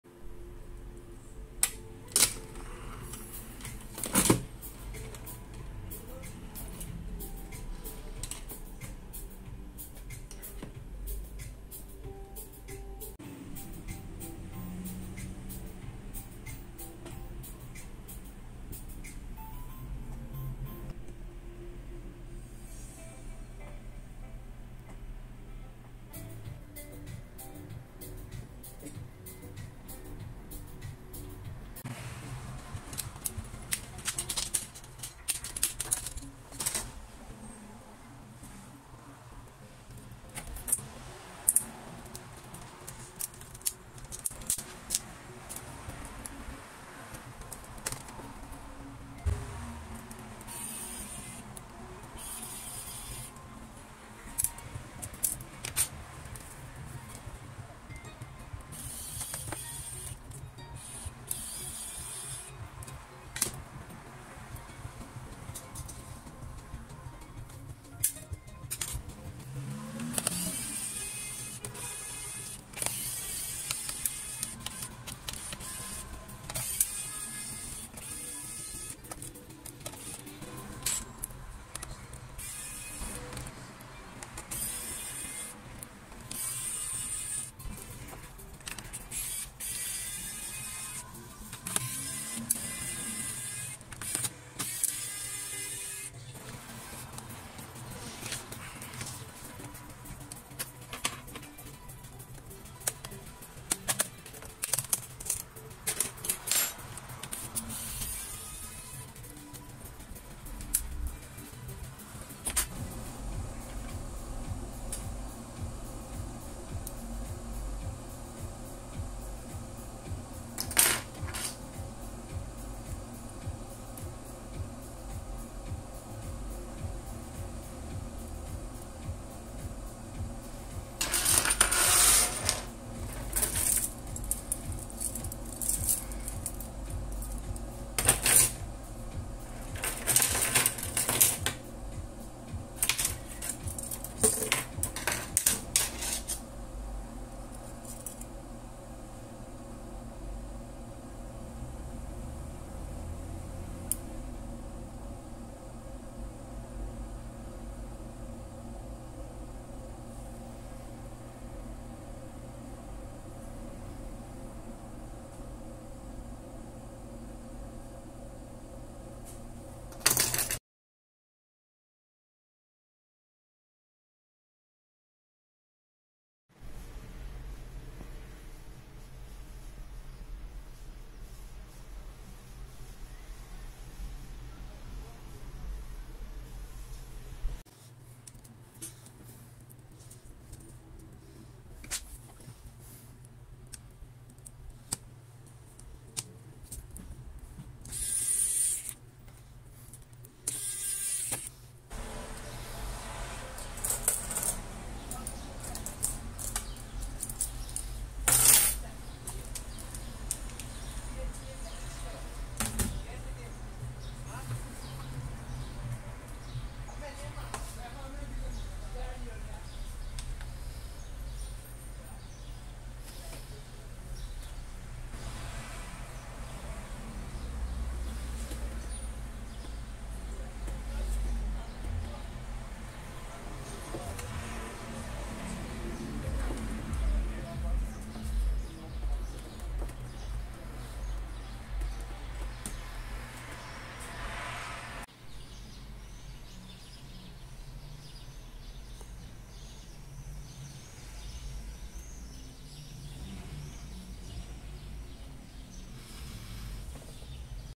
Repair broken Fujitsu notebook
restore broken notebook and lcd replacement